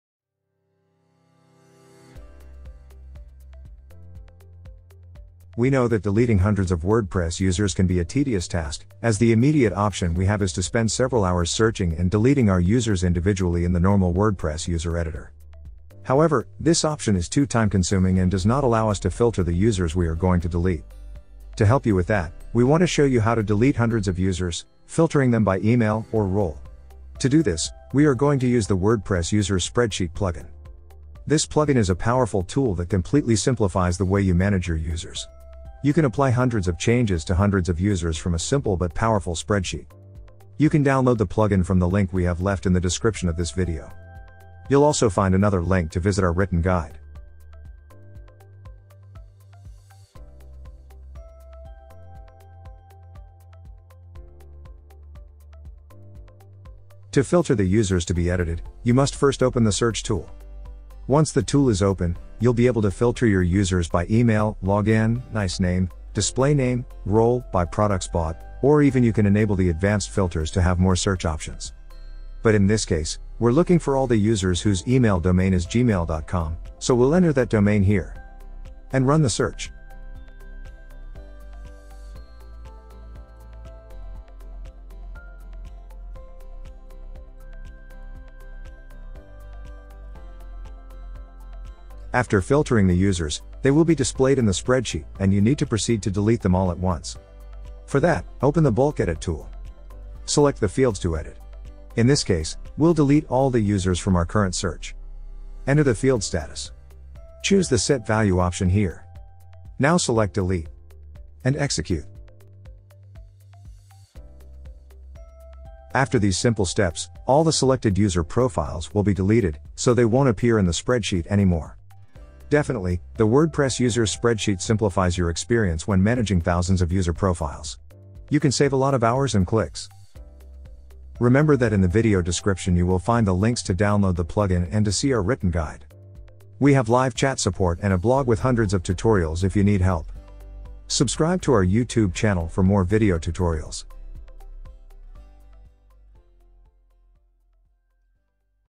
WordPress – Delete Thousands of Users by Email, Keyword, Role
In this video you'll learn how to filter your users by email, keyword, or role in order to delete all search results at once using the powerful bulk editing tool that the WP Sheet Editor plugin offers.

00:59 How to filter the users to be deleted

01:26 How to delete all filtered users at once